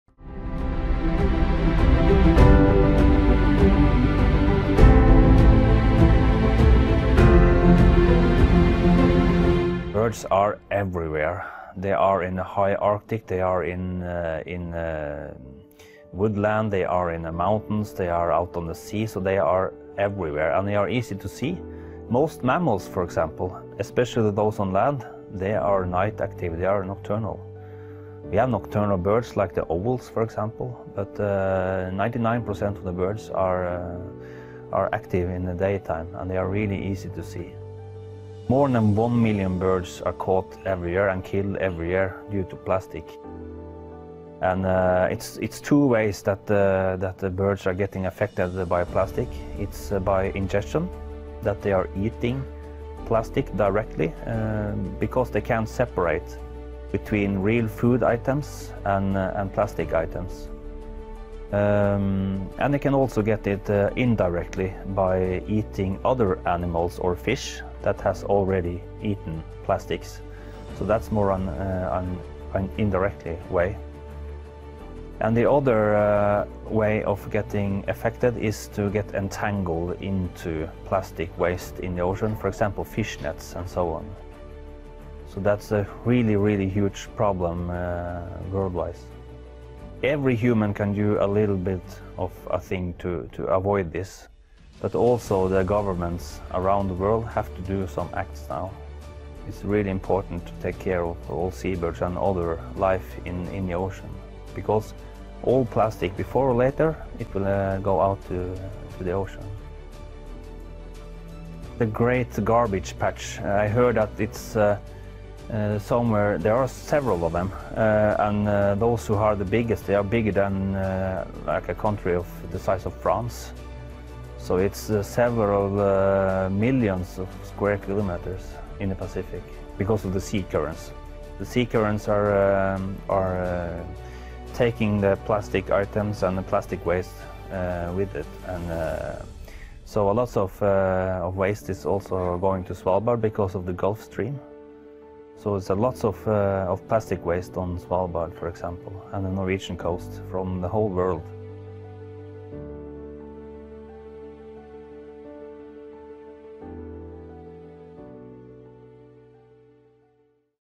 Arctic cruise with Ola Nordsteien, Bird Researcher, Faces of the Polar World
While penguins are considered the signature birds of Antarctica, choosing one representative for the Arctic is not an easy task. The great variety of endemic species are annually joined by thousands of migratory birds, all unique and beautiful in their own way. Expedition cruises to the High Arctic are the best way to experience the abundant and fragile birds of the polar world.

On this Arctic voyage, we were lucky to have Ola Nordsteien on board, a Norwegian bird researcher from the Jomfruland Bird Observatory. He shares his concerns about plastic pollution negatively affecting Arctic birds. Plastic waste accumulates rapidly and travels with the ocean currents, taking decades to break down. It poses a great threat to seabirds and other wildlife and, needless to say, does not belong in the picturesque landscapes of the wild islands and ocean basins.

It’s up to all of us to take care of the polar ecosystem and wildlife to keep them pristine for future generations. Adjusting your daily plastic use and raising awareness are the easy steps that can be done. There are also ways to take action. In Svalbard, Poseidon Expeditions takes part in the “Clean Up Svalbard” initiative, reducing beach waste on each of our landings. This program is coordinated by AECO, the Association of Arctic Expedition Cruise Operators. Joined by our passengers, we pick up the debris from the beach and later deposit it in a dedicated bin in Longyearbyen, where it will be properly processed.